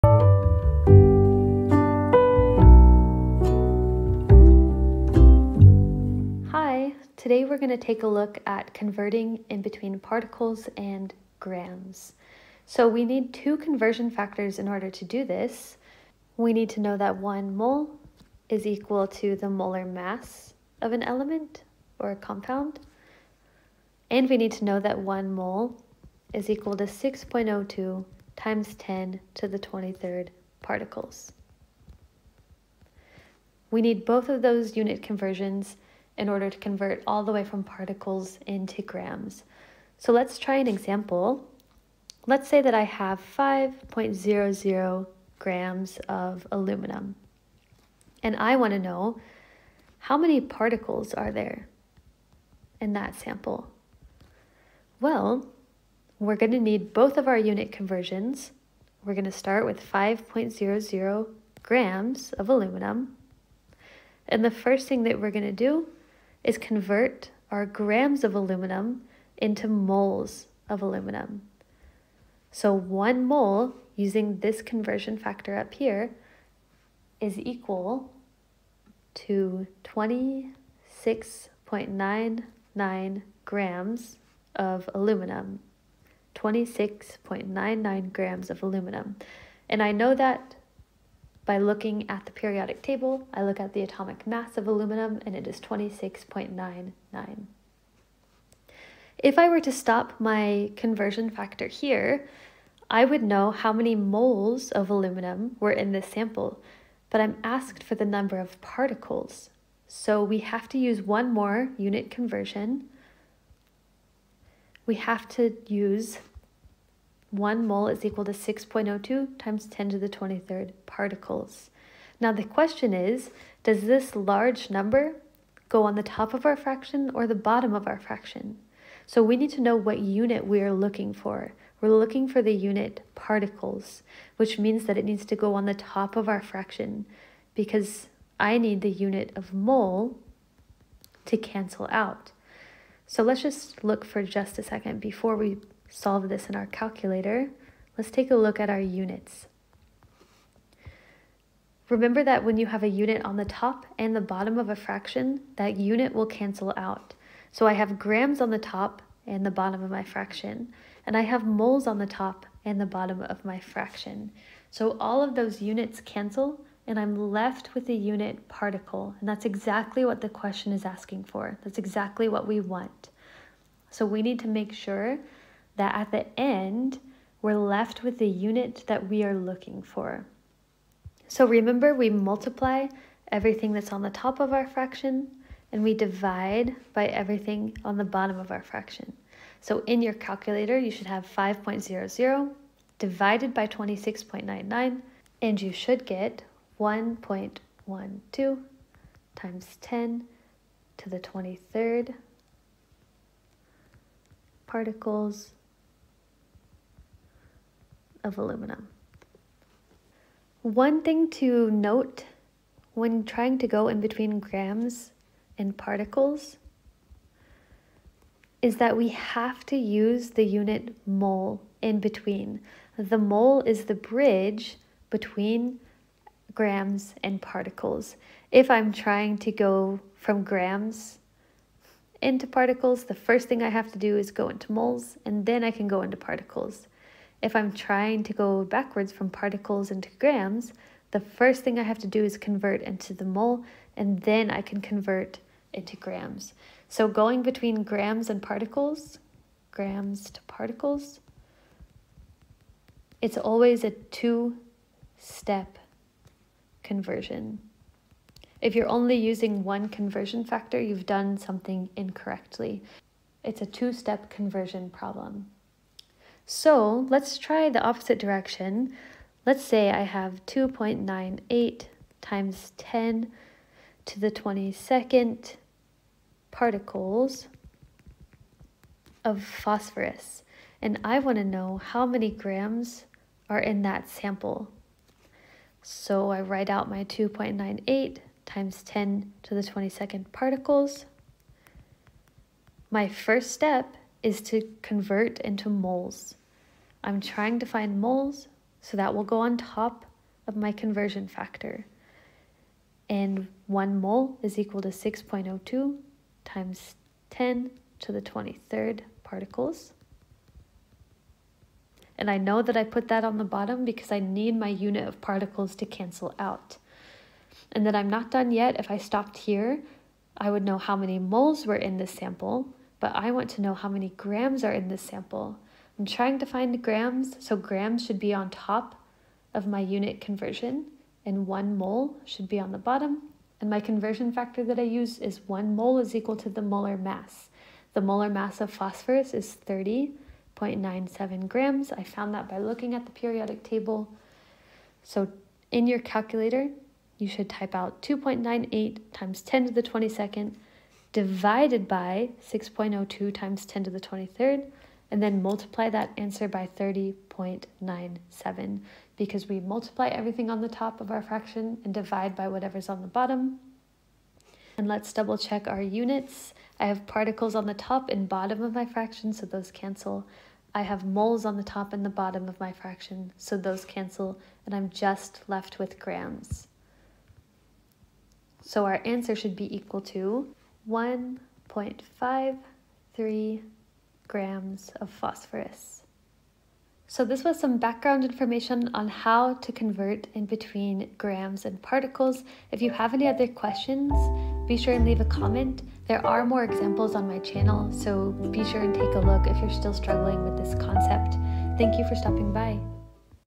Converting Between Particles and Grams
This video goes over how to convert the number of particles of a substance into grams. This is a great review, as well as a great resource if you are struggling with this topic!